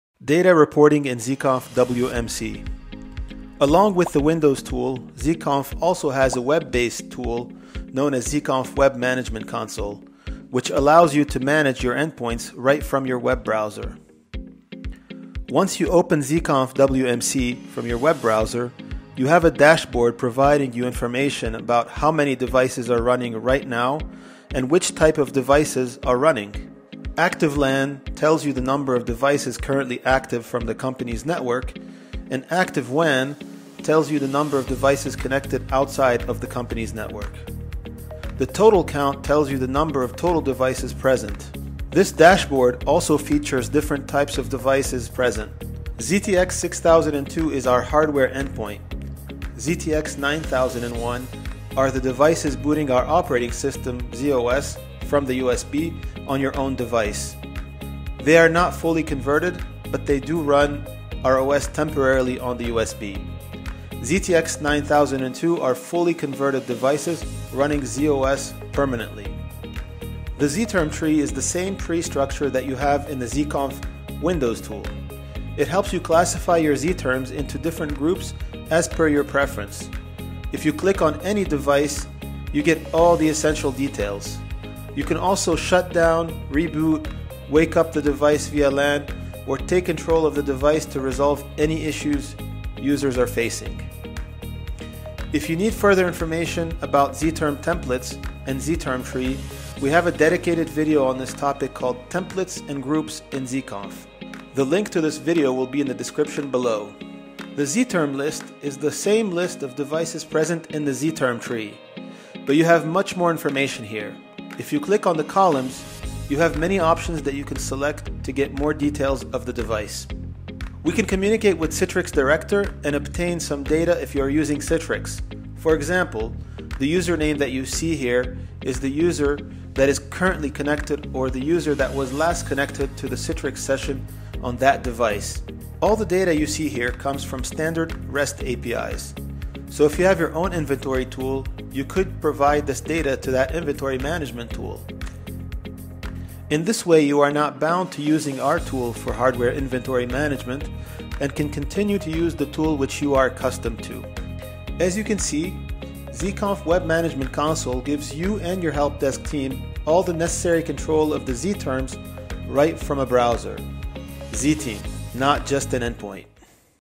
Data reporting in ZeeConf Web Management Console
This video explains the dashboard & all the basic features of ZeeConf Web Management Console.

Below it the link for Templates and Groups in ZeeConf.